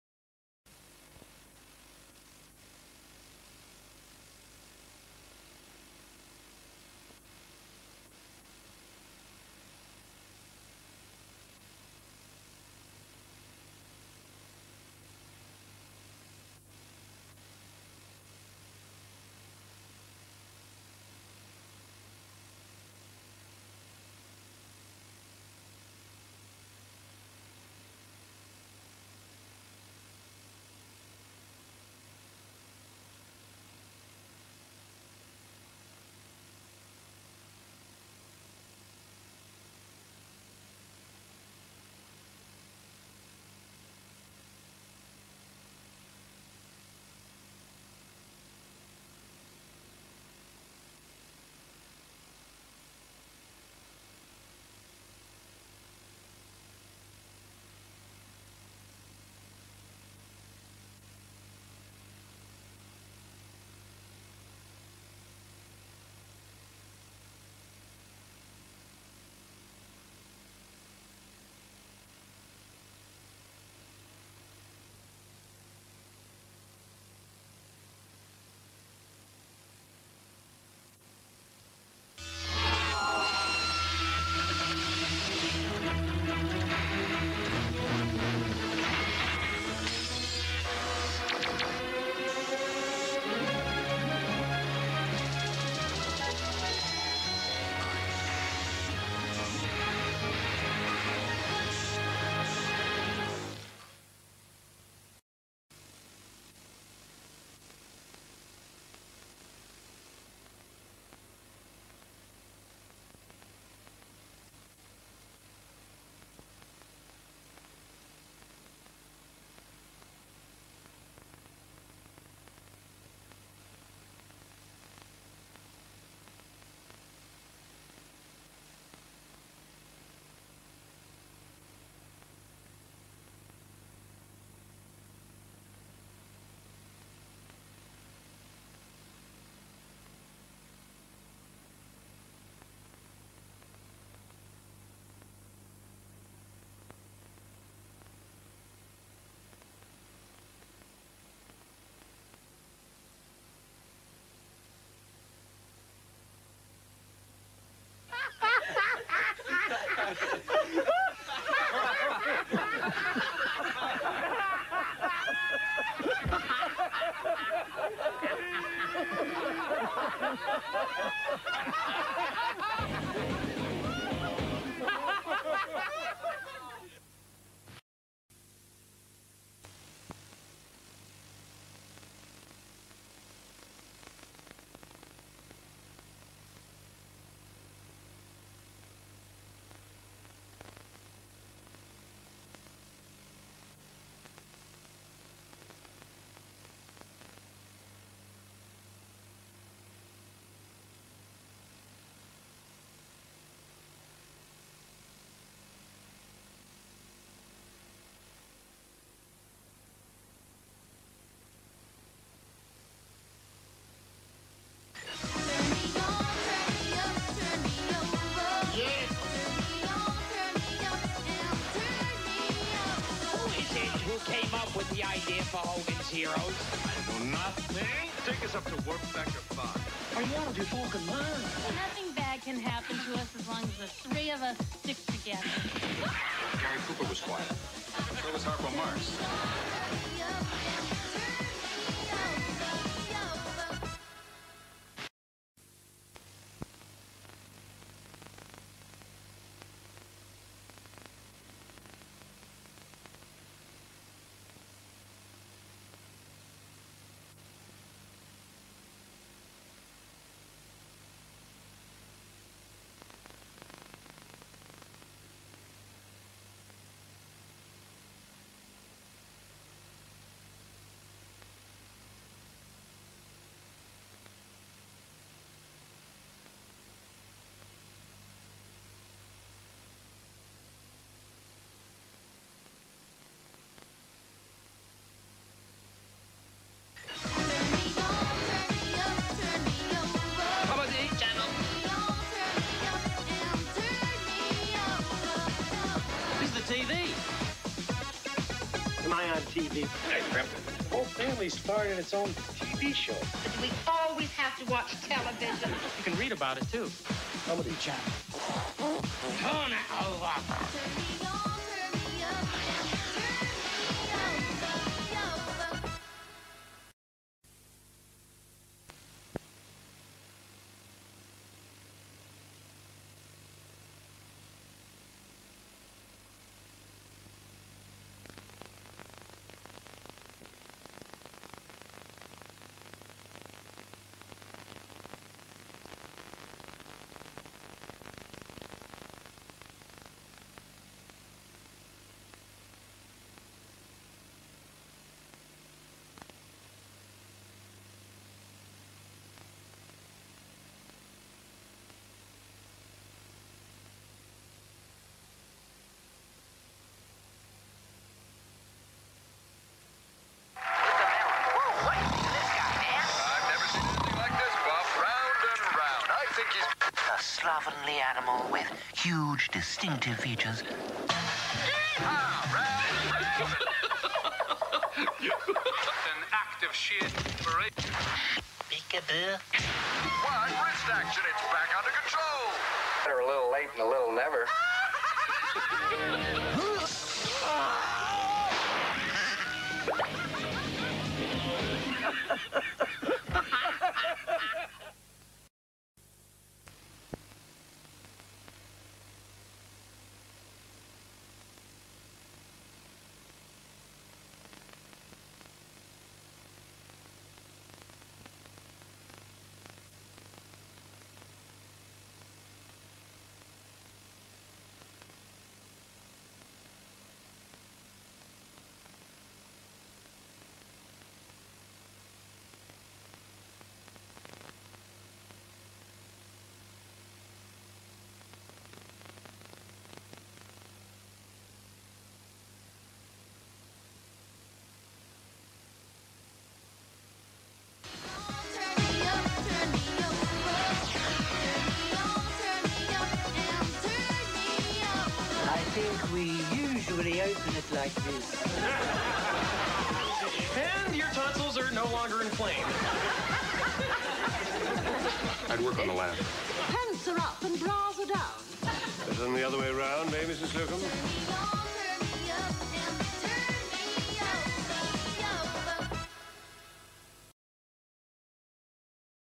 The Comedy Channel Testcard & Startups 1992
Some startups from the short-lived Comedy Channel including the fishtank based test card and various trails